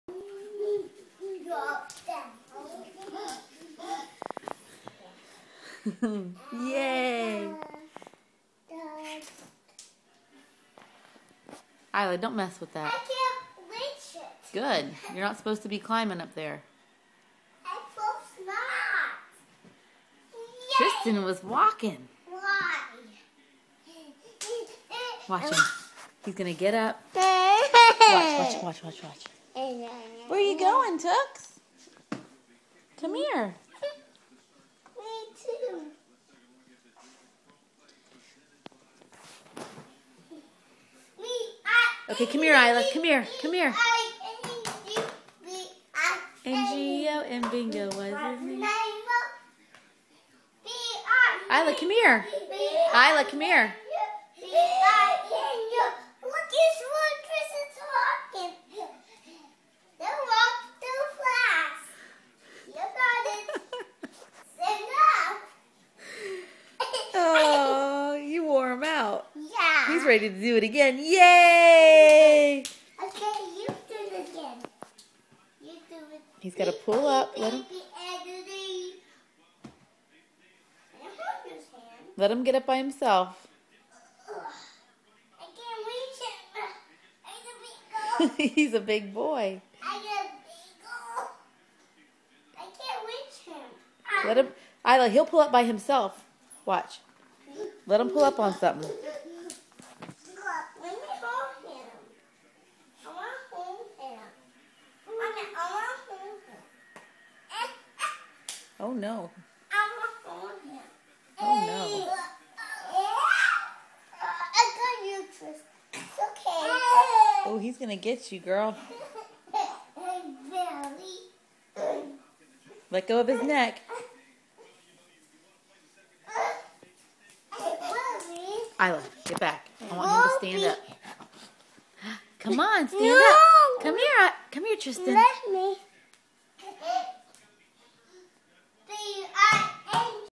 Brother and sister
Isla helping Tristan walk